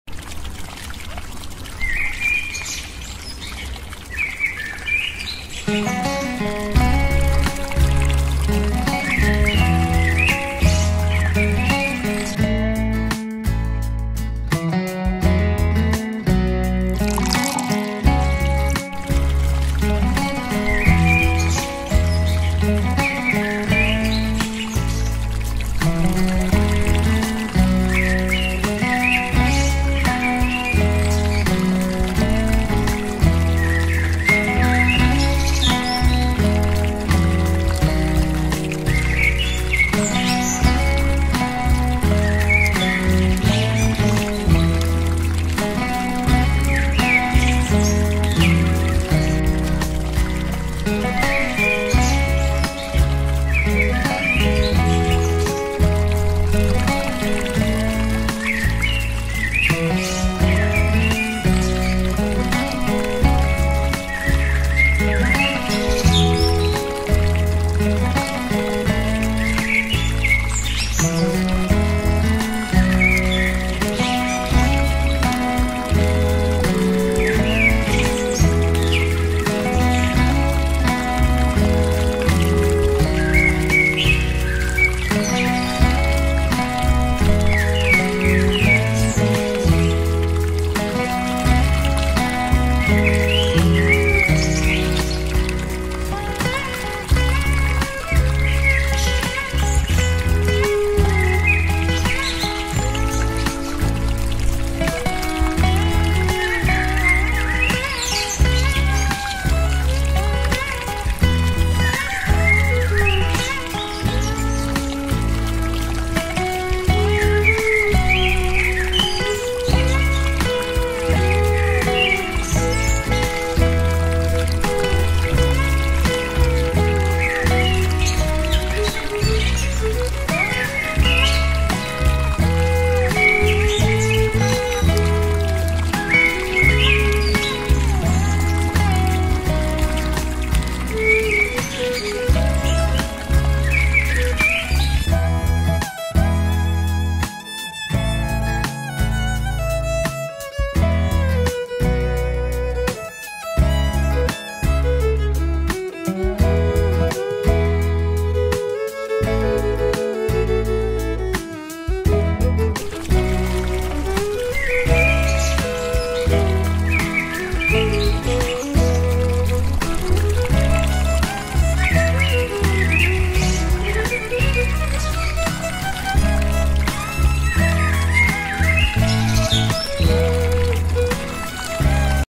Countryside rustic food: surprised by the delicious taste of traditional grilled chicken specialty.
Rice cooked in bamboo tubes and super fragrant charcoal grilled chicken, Countryside food